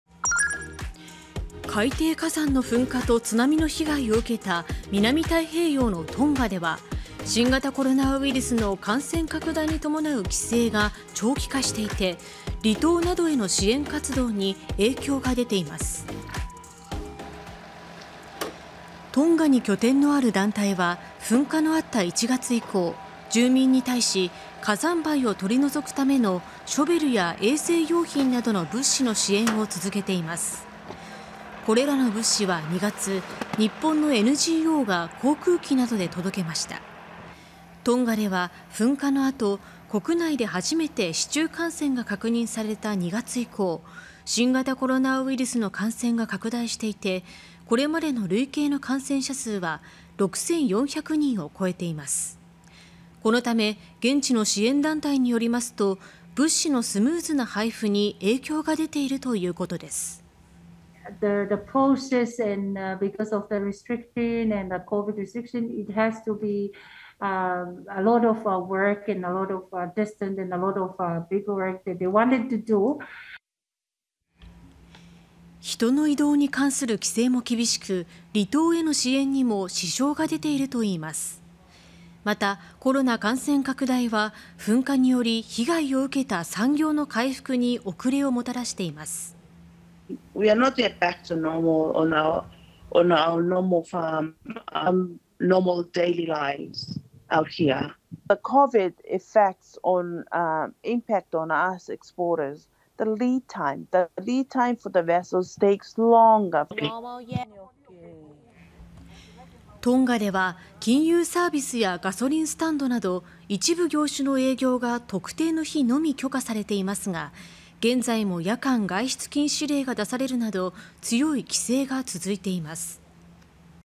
噴火と津波被害のトンガ 新型コロナ感染拡大で支援に影響(2022年4月5日)
海底火山の噴火と津波の被害を受けた南太平洋のトンガでは、新型コロナウイルスの感染拡大に伴う規制が長期化していて、離島などへの支援活動に影響が出ています。

　トンガに拠点のある団体は、噴火のあった1月以降、住民に対し火山灰を取り除くためのショベルや衛生用品などの物資の支援を続けています。

　これらの物資は2月、日本のNGO（非政府組織）が航空機などで届けました。

　トンガでは噴火の後、国内で初めて市中感染が確認された2月以降、新型コロナウイルスの感染が拡大していて、これまでの累計の感染者数は6400人を超えています。

　このため、現地の支援団体によりますと、物資のスムーズな配布に影響が出ているということです。

　現地支援団体TCDT・カロ氏：「距離を確保したり書類を作ったりと、コロナ規制で手続きに多くの手間がかかるようになりました」

　人の移動に関する規制も厳しく、離島への支援にも支障が出ているといいます。

　また、コロナ感染拡大は、噴火により被害を受けた産業の回復に遅れをもたらしています。

　農産物の輸出業者・メレ氏：「私たちの仕事や日常生活は今も戻っていません。新型コロナの影響は輸出業者も受けています。（商品が輸出されるまでに）とても時間がかかってしまうんです」

　トンガでは金融サービスやガソリンスタンドなど一部業種の営業が特定の日のみ許可されていますが、現在も夜間外出禁止令が出されるなど、強い規制が続いています。